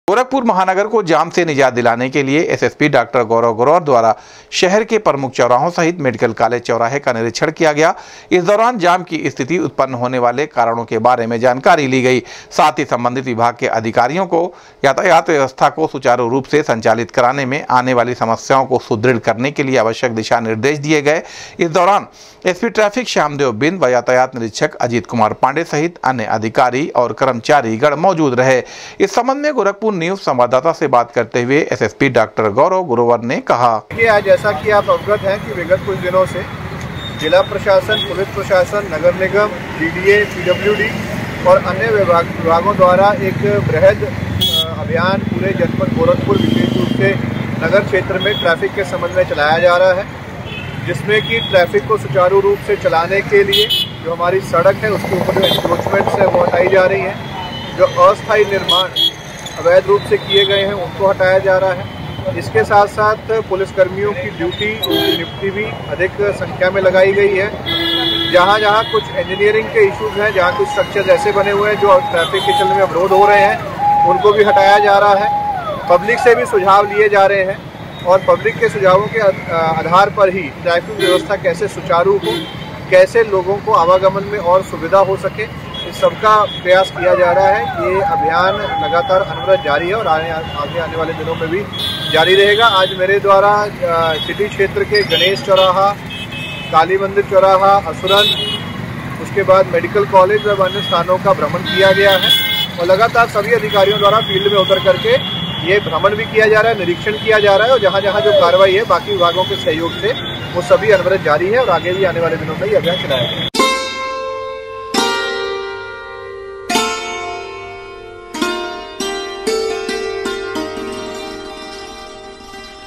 एसएसपी डॉ० गौरव ग्रोवर द्वारा शहर के प्रमुख चौराहो सहित मेडिकल कॉलेज चौराहे का किया गया निरीक्षण
गोरखपुर महानगर को जाम से निजात दिलाने के लिए एसएसपी डॉ० गौरव ग्रोवर द्वारा शहर के प्रमुख चौराहो सहित मेडिकल कॉलेज चौराहे का किया गया निरीक्षण, संबंधित विभाग के अधिकारी व कर्मचारी गण को दिए आवश्यक दिशा निर्देश